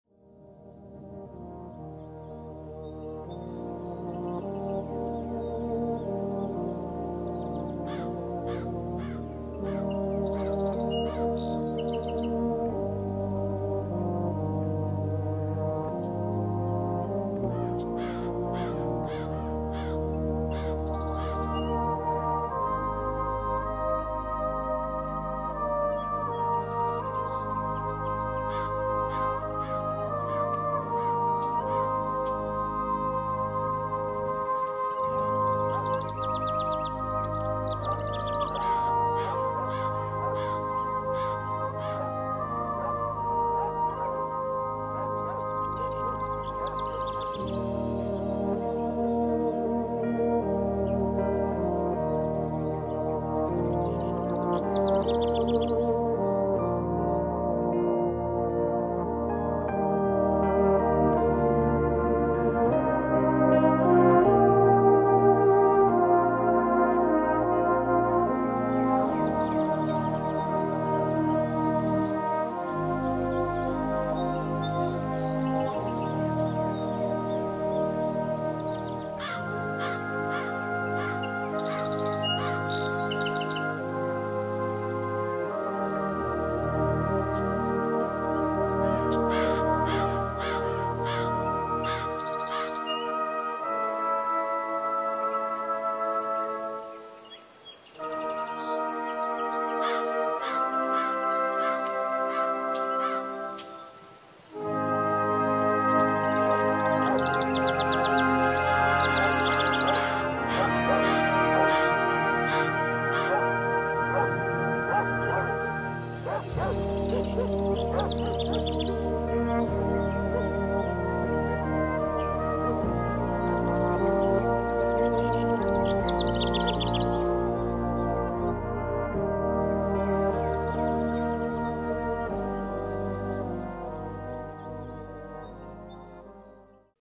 CRICH STAND MEMORIAL SD 480p
The Tower is dedicated to the memory of the soldiers from the Sherwood Foresters who died in Word War I and World War II.